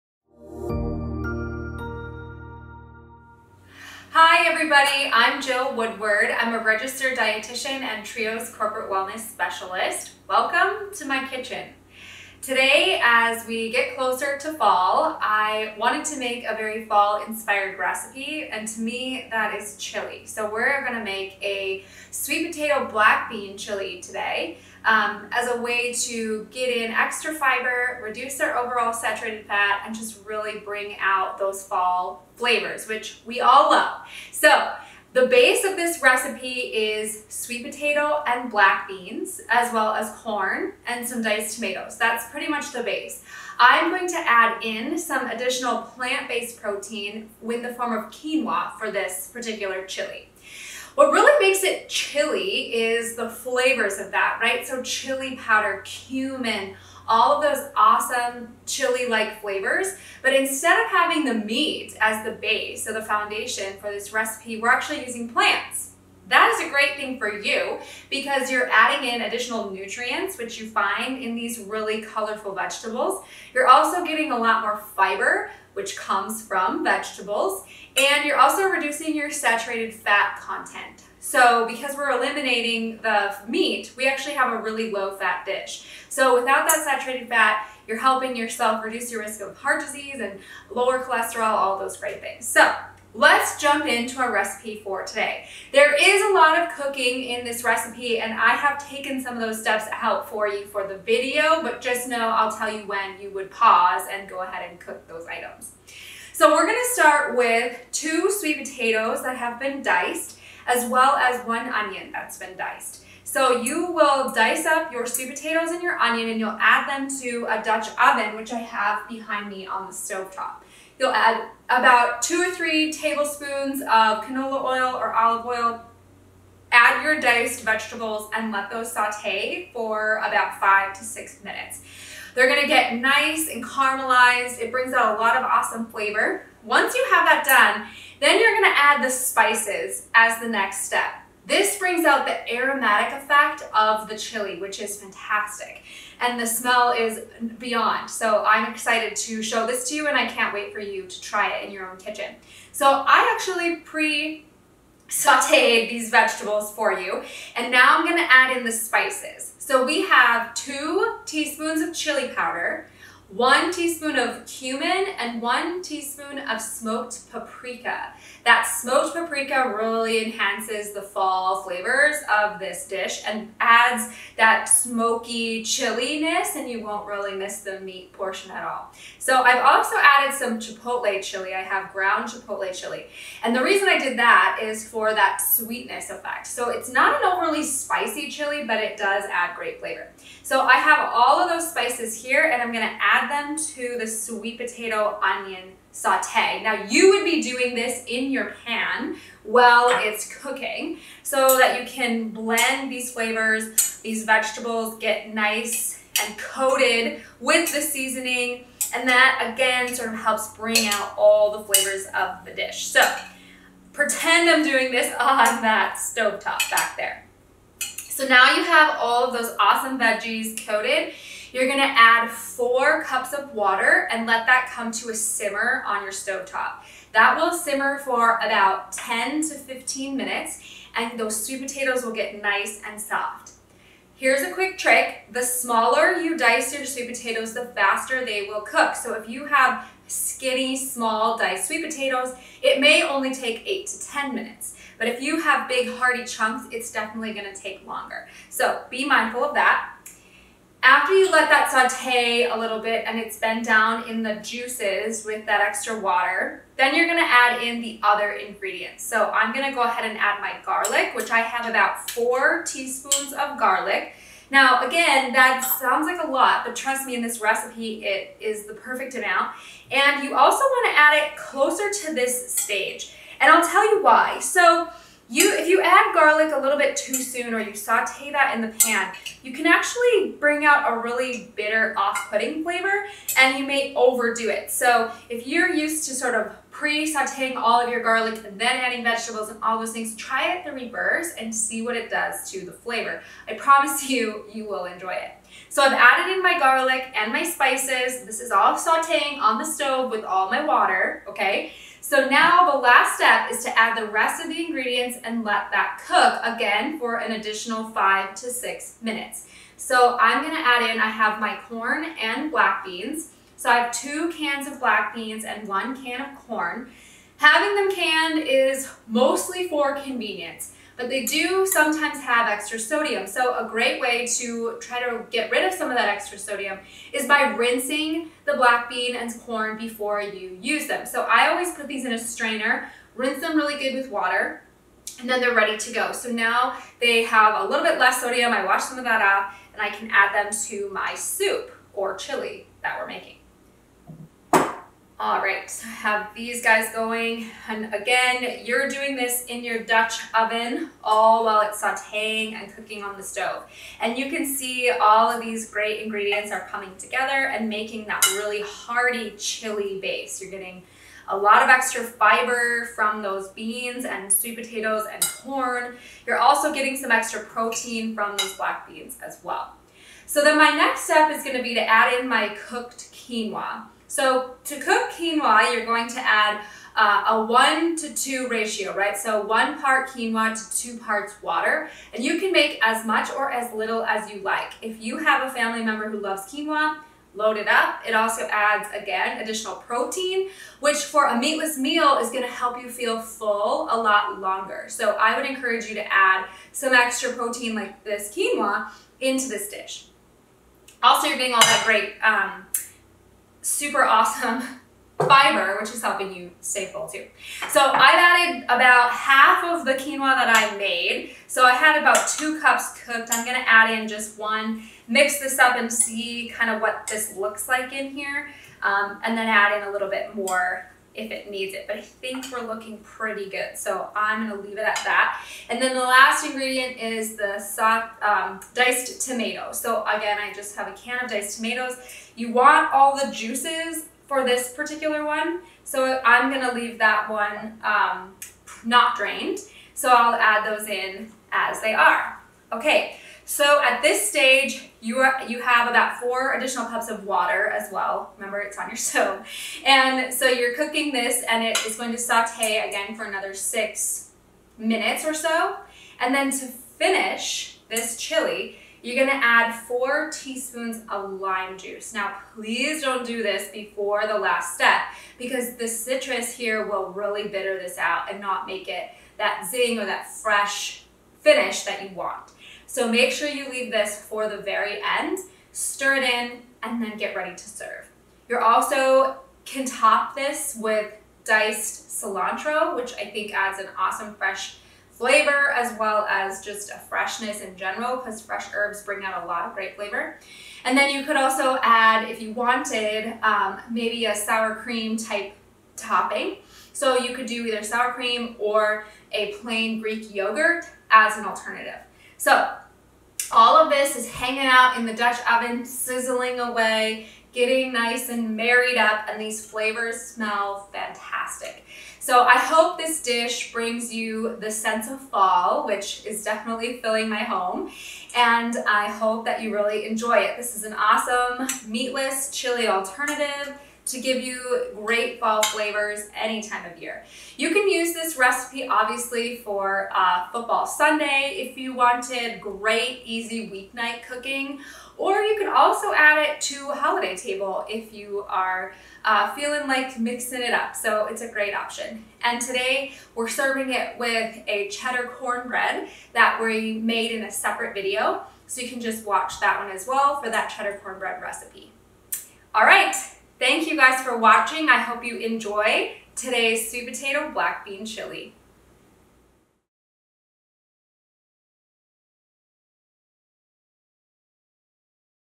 Sweet Potato & Black Bean Chili with Treo Wellness
Fall is filled with delicious flavors and many food focused events! Spice up your fall with this healthful fall spiced sweet potato & black bean chili. All these vegetables provide vitamins, minerals, fiber, and antioxidants to improve your nutrition all while bursting with flavor!

Join Treo Wellness and Registered Dietitian Jill Woodward as we make this new fall favorite!

Recipe
3 tbsp oil
2lb sweet potatoes, peeled & diced
1 large onion, diced
2 (15oz) cans black beans, rinsed and drained
1 (14oz) can diced tomatoes (low sodium if possible)
1 cup corn kernels
4 cloves garlic, minced
1/2 tsp ground chipotle chile
1/2 cup chopped fresh cilantro
4 tsp lime juice
2 tsp chili powder
1 tsp ground cumin
1 tsp smoked paprika
1 tsp salt & 1/4 tsp black pepper
2-2 1/2 cups water

What you do:
1. Heat oil in a dutch oven over medium high heat. Add sweet potatoes, stirring often, until the onion is beginning to soften, about 4 minutes.
2. Add all spices and cook, stirring constantly, for 30 seconds
3. Add water and bring to a simmer. Cover, reduce heat to maintain simmer and cook until the sweet potato is tender, 10-12 minutes
4. Add beans, tomatoes, corn, cook for 5 minutes.
5. Add lime juice, and top with cilantro for serving.

*can add greek yogurt or sour cream as a topping